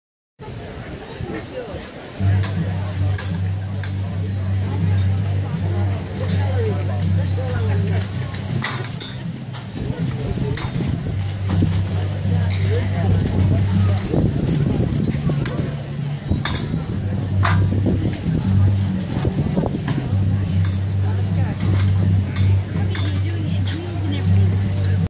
Bulldog Folding Trailer - So easy to use!
The Bulldog Folding Trailers are so easy to fold. Take a look, this is me - doing it! A WOMAN! Its light weight, easy to handle and the folding and unfolding is very simple.